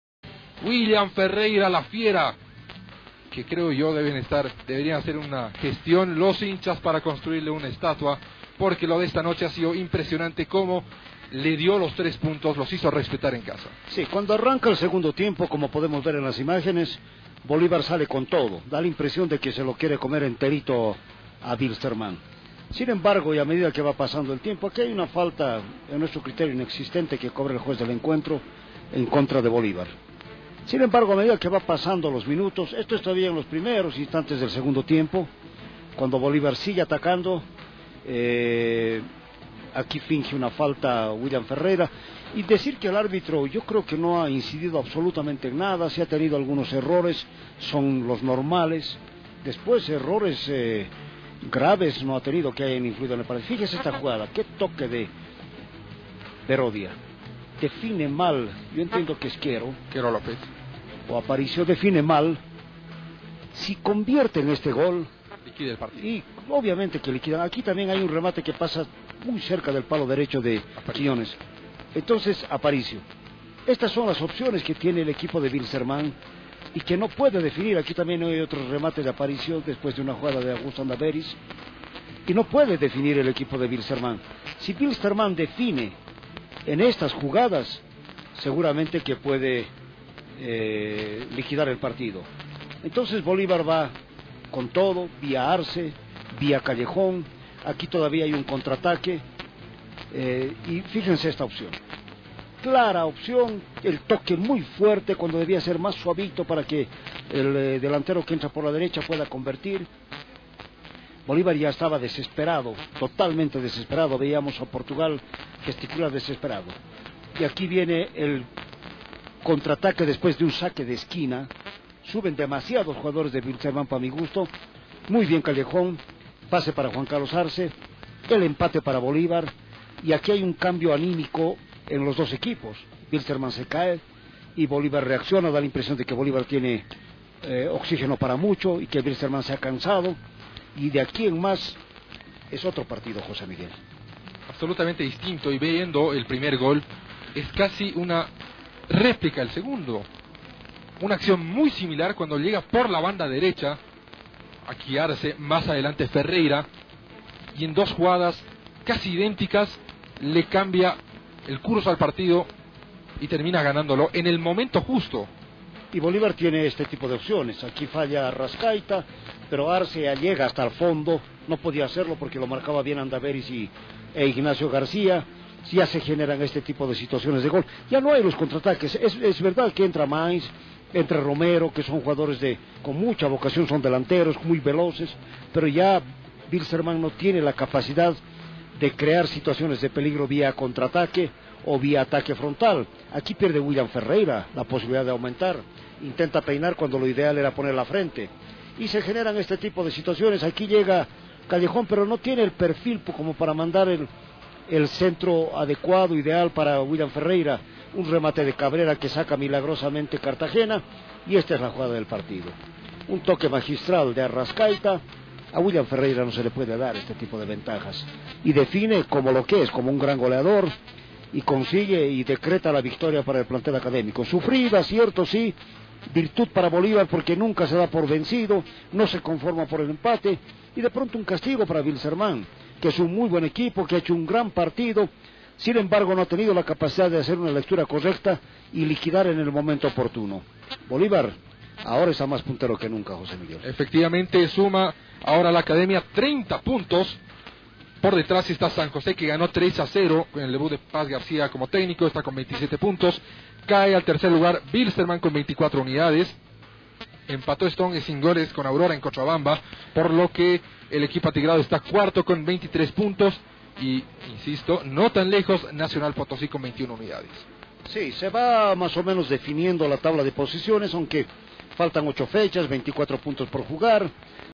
BOLÍVAR 2 - Wilstermann 1: Torneo Apertura 2013/2014
La Academia remonta el marcador y vence en el último suspiro del compromiso de la Decimo Cuarta Fecha, Wilstermann vino a defenderse con uñas y dientes pero esta vezB los jugadores de la Academia pudieron sobrepasar la barrera impuesta por el rojo del valle, los goles para Bolívar fueron convertidos por Juan Carlos Arce y William Ferreira